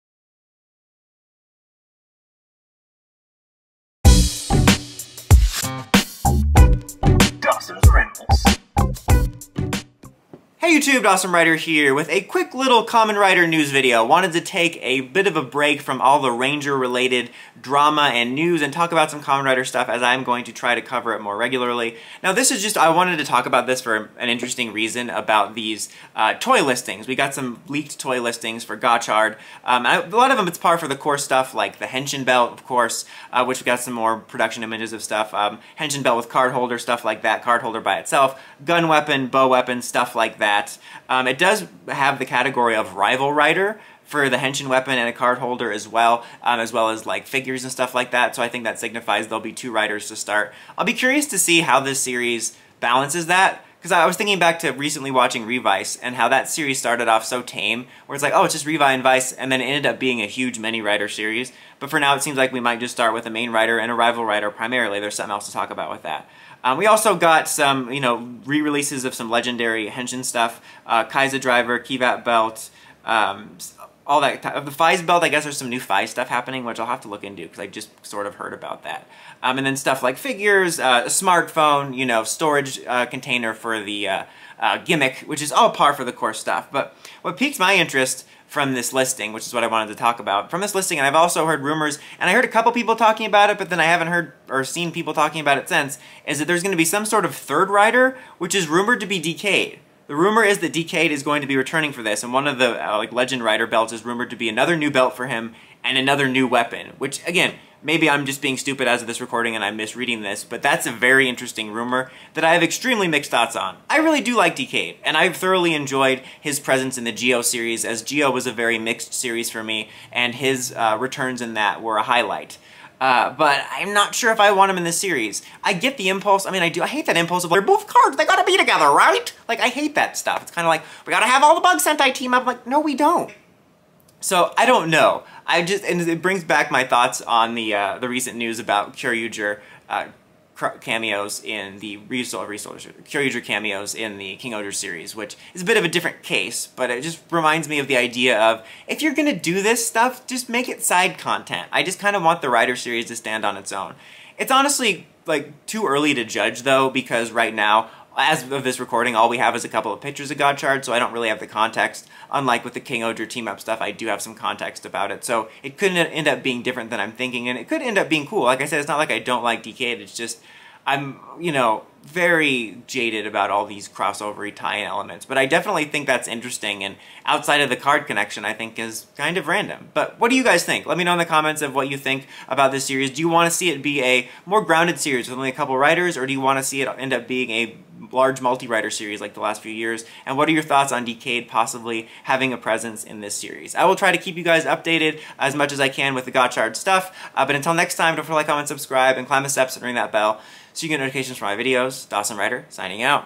Kamen Rider Decade To Return For Gotchard
We got some toy listings for the upcoming Gotchard series that reveal a rival rider, and the possible return of Kamen Rider Decade?